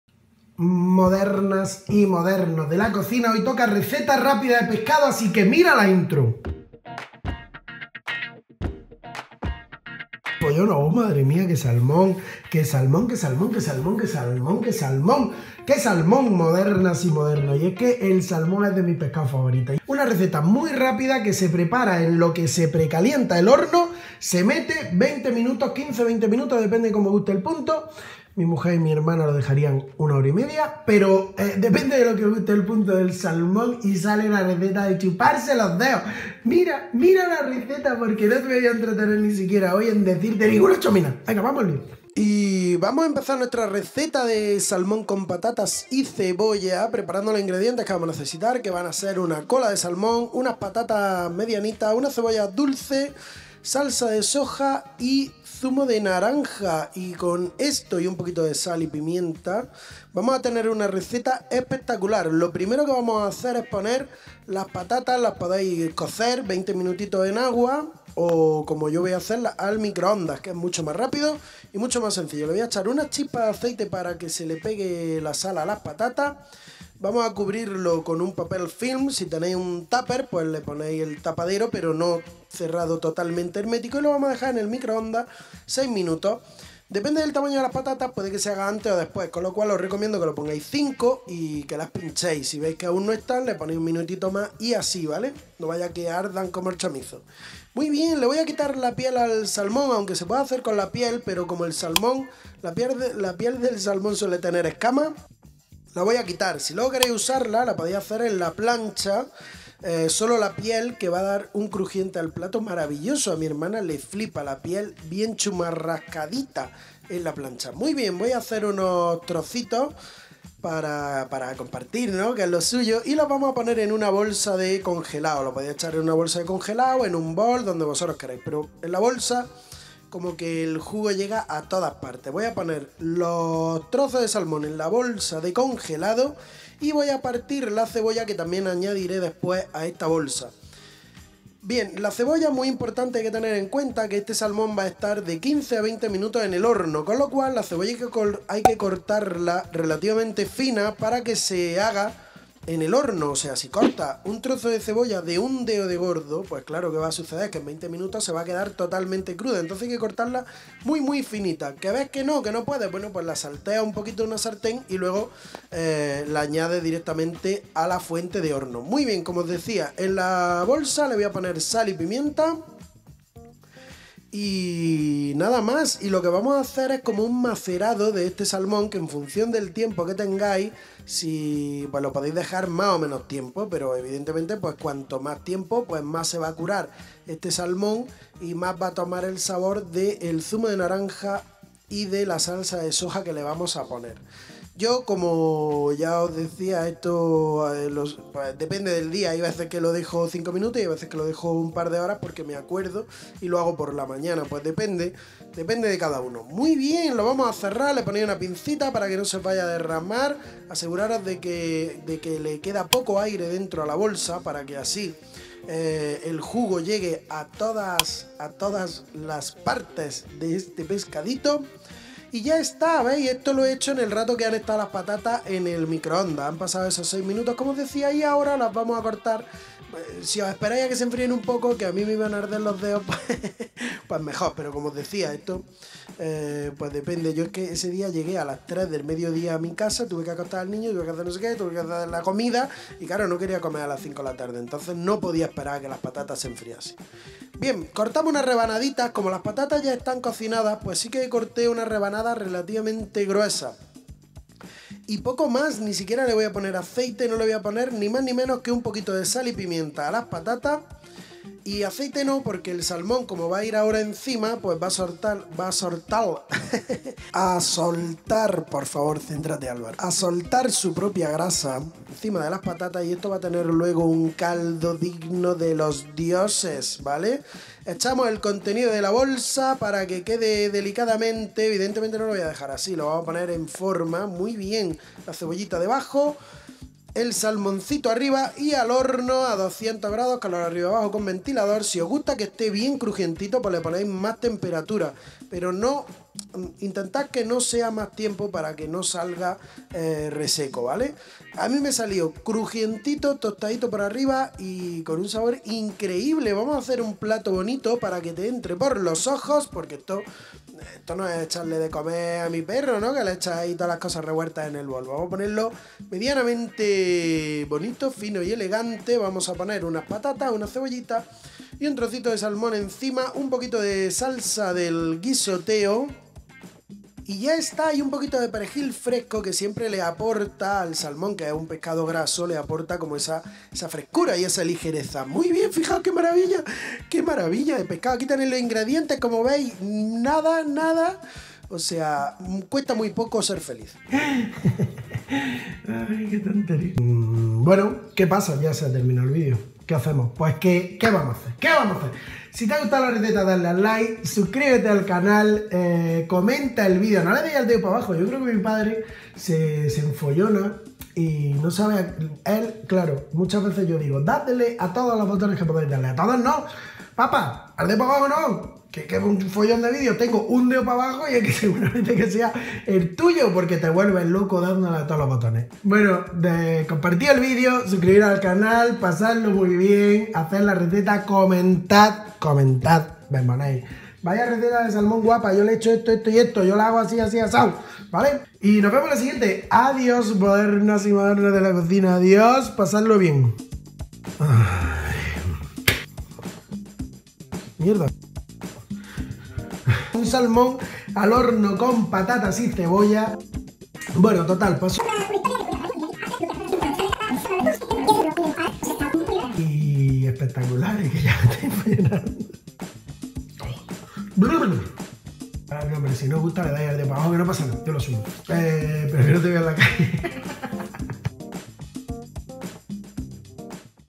SALMÓN al HORNO con patatas y cebolla | Fácil y Rápido
Receta de Salmón al horno con patatas y cebolla muy fácil y rápido

y lee o imprime la receta paso a paso con los ingredientes y consejos

Hola que tal modernas y modernos!!! El otro día me dieron las 15:00 cuando llegué a casa y aún tenía que acostar al niño y eso ya te digo que se lleva un rato. Así que decidí preparar una receta super express.
Una receta de salmón al horno muy fácil y rápida pero con un sabor extraordinario y es que para mi de las mejores mezclas que hay, son el salmón con la salsa de soja y el zumo de naranja me parece una combinación genial.
Os dejo esta receta sacada de la manga y fruto de la desesperación y el hambre y como siempre puedes ver la receta en vídeo o leerla tranquilamente.

DISFRUTAD y gracias por Leerme!!!

Puedes elegir entre una enorme variedad de cuchillos y buscar el que se adapte a tus necesidades.